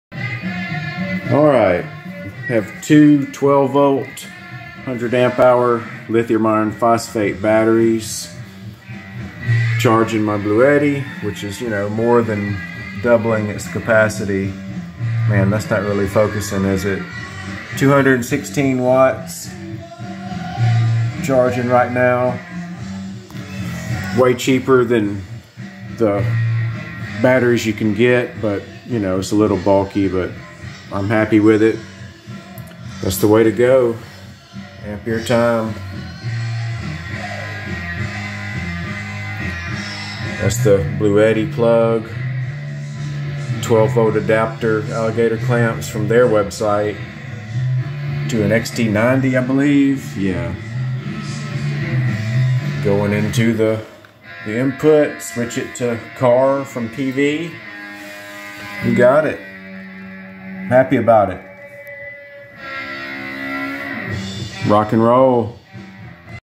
Charging Bluetti AC200p with 24 volts. Bluetti Ampere Time
two 12 volt 100 amp hour batteries, wired in series. charging the Bluetti ac200p with 24 volts, more than doubling its capacity. Good Times